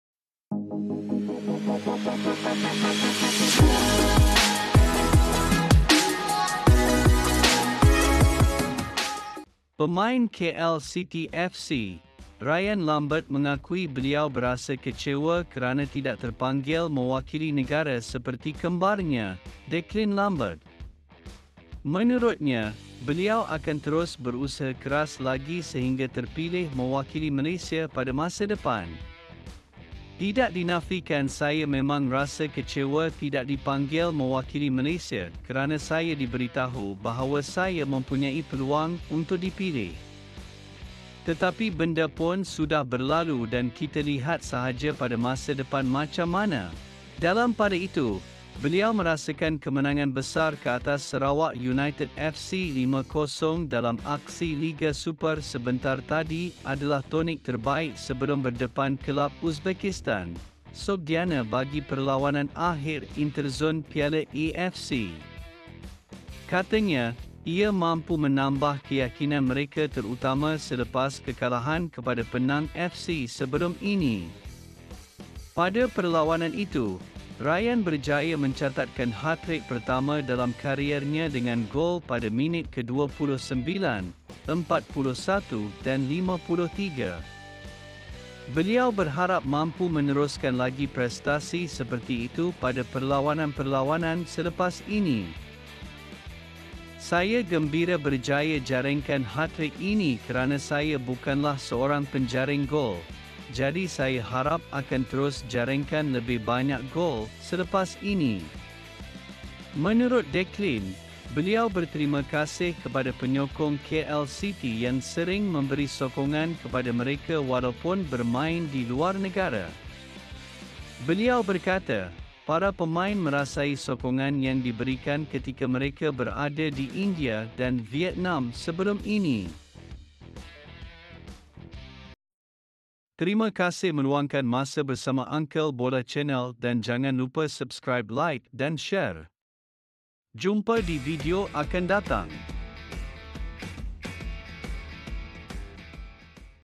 Ryan Lambert kecewa kerana tidak terpanggil mewakili negara ?
Pemain KL City FC, Ryan Lambert mengakui beliau berasa kecewa kerana tidak terpanggil mewakili negara seperti kembarnya, Declan Lambert.

Menurutnya, beliau akan terus berusaha keras lagi sehingga terpilih mewakili Malaysia pada masa depan.

CREDIT : @Unifi.YourWorld
Uncle Bola

Channel Cerita Bola, Berita Bola, Makan Bola dan Semua Tentang Bola.